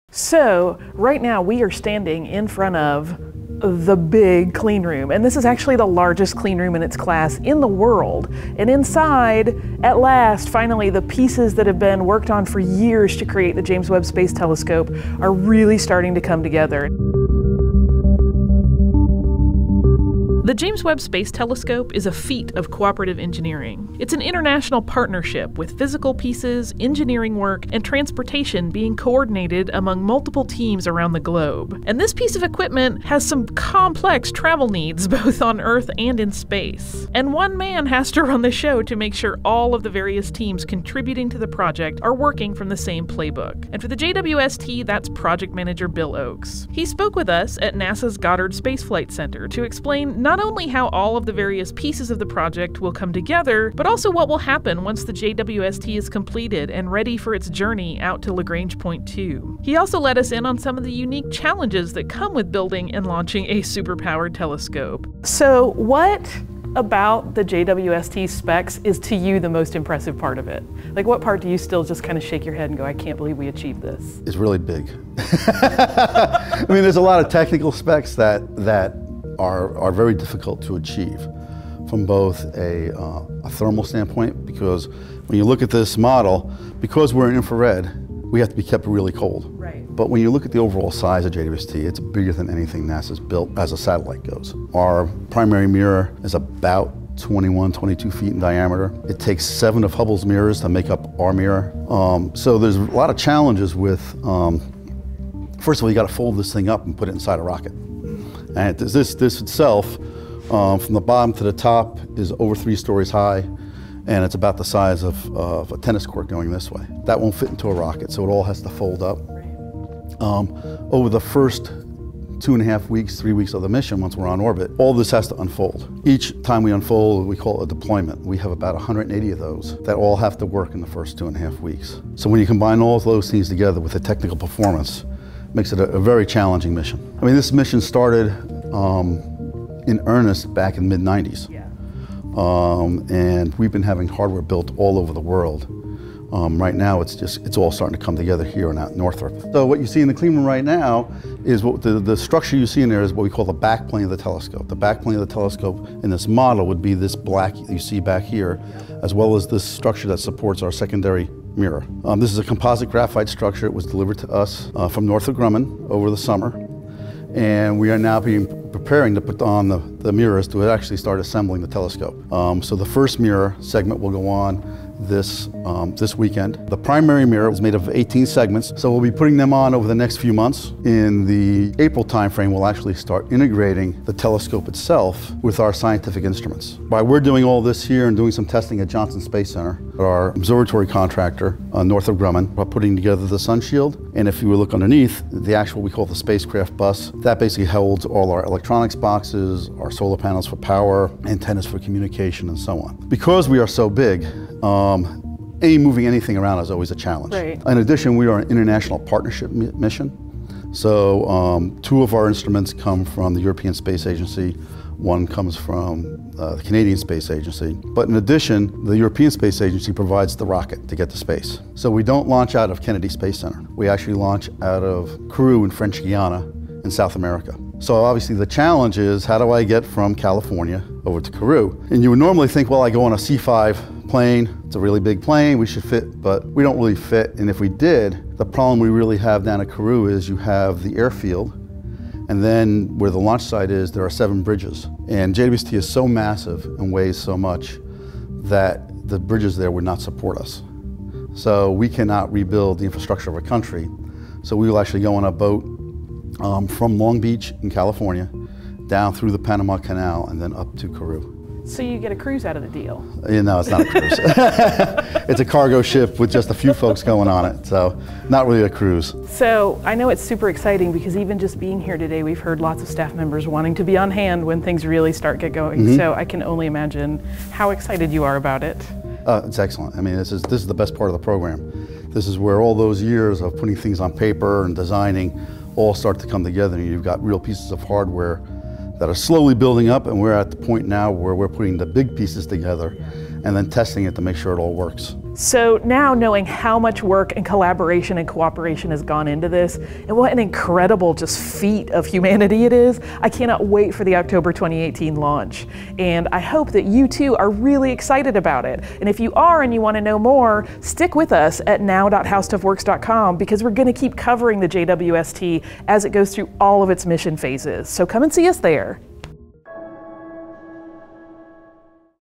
Building an Epic Telescope | HowStuffWorks NOW
2/19/2016: The James Webb Space Telescope is the product of massive efforts from multiple different teams around the globe. How does a project like that get organized, and who’s in charge of keeping everyone on track?

Special thanks to the entire team at NASA’s Goddard Space Flight Center for hosting our visit, to Bill Ochs, Dr. John Mather, Begoña Vila, Jody Davis and Dr. Stefanie Milam for sharing their time, knowledge and enthusiasm with us, to Laura Betz for managing our time on site, and to  Maggie Masetti for making sure the entire shoot ran smoothly and serving as science consultant.